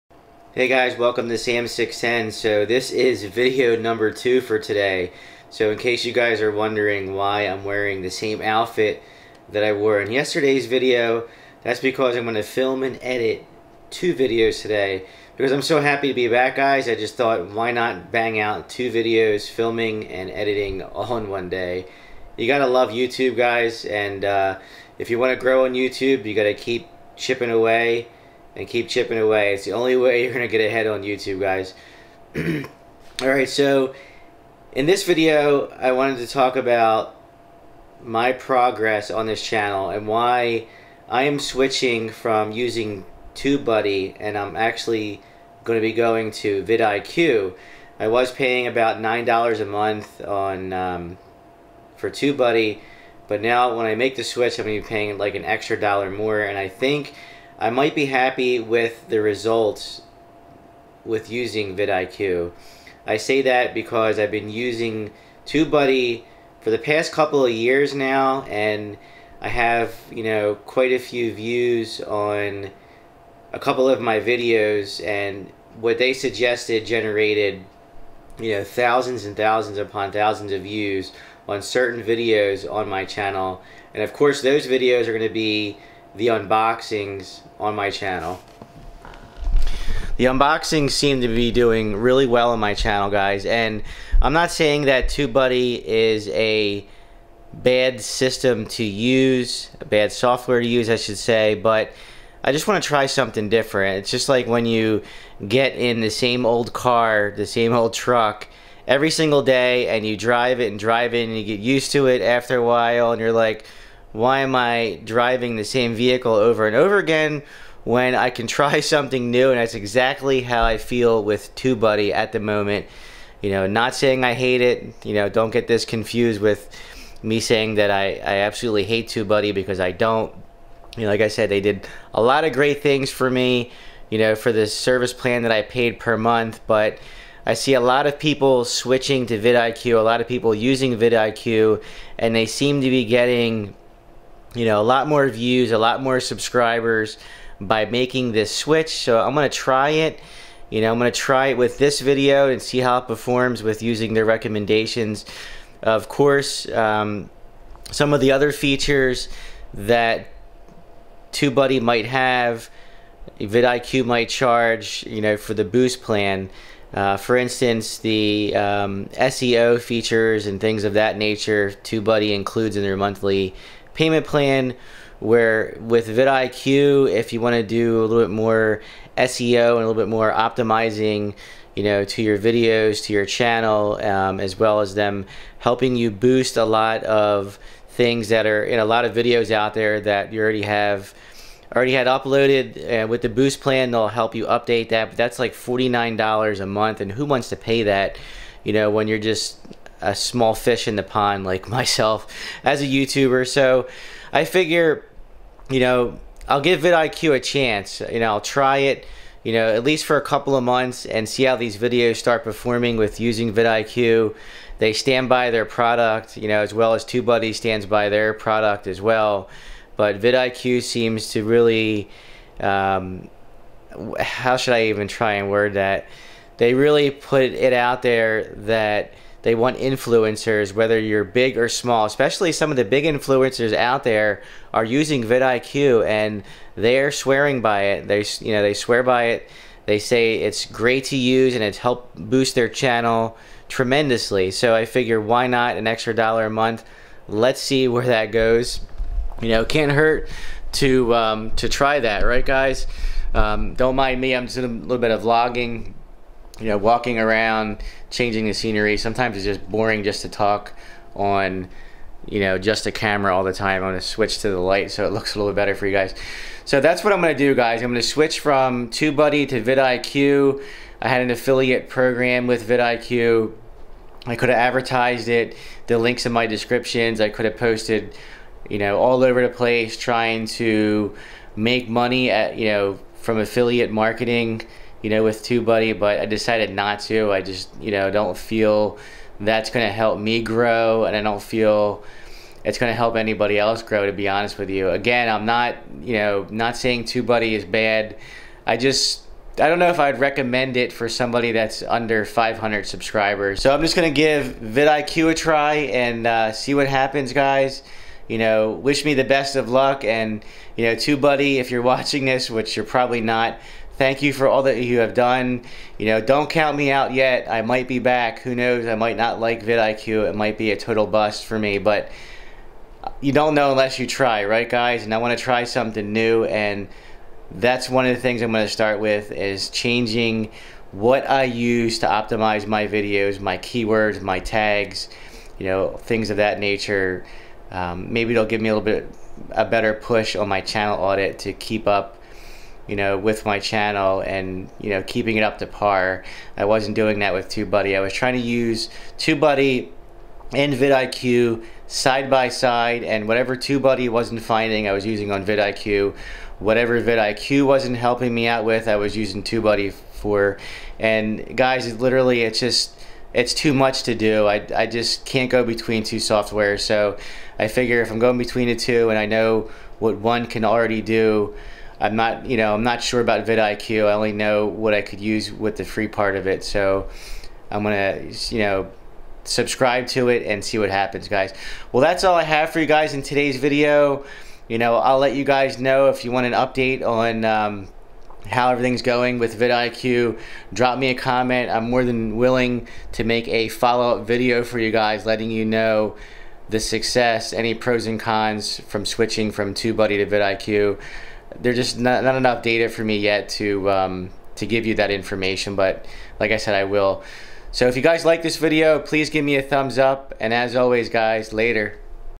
I Switched From Tubebuddy To vidIQ
I Switched From Tubebuddy To vidIQ - In this video, I wanted to explain to you why I switched from TubeBuddy to vidIQ. TubeBuddy was really helpful for me my last 2 years grinding out YouTube videos. My thought process is that vidIQ can offer me a wider range of video optimization.